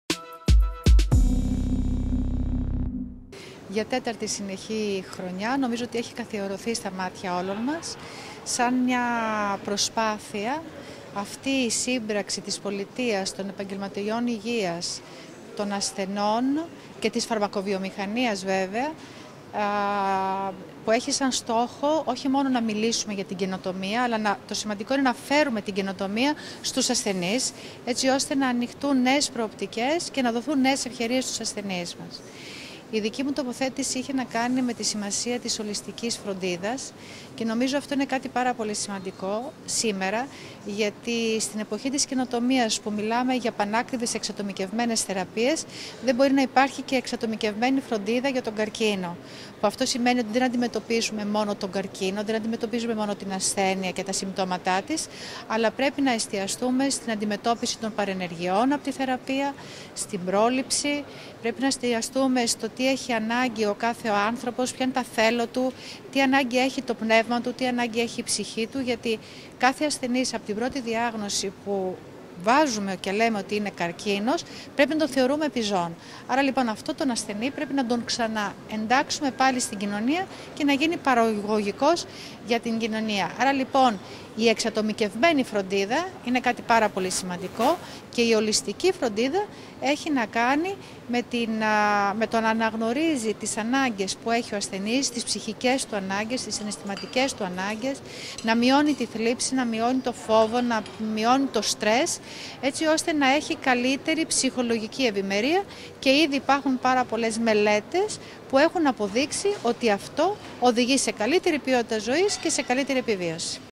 Economist-Greece’s determination to fight cancer-A. Christopoulou St. Andrew’s Hospital of Patra
Η Athina Christopoulou, Medical oncologist, head, Department of Internal Medicine, Division of Clinical Oncology, St. Andrew’s General Hospital of Patra, μίλησε στο ΑΠΕ-ΜΠΕ σχετικά με τη συμμετοχή του στο συνέδριο Greece’s determination to fight cancer, που διοργάνωσε ο Economist Impact στο Athenaeum InterContinental Athens Hotel , στην Αθήνα στις 12 Μαρτίου. Πηγή ΑΠΕ-ΜΠΕ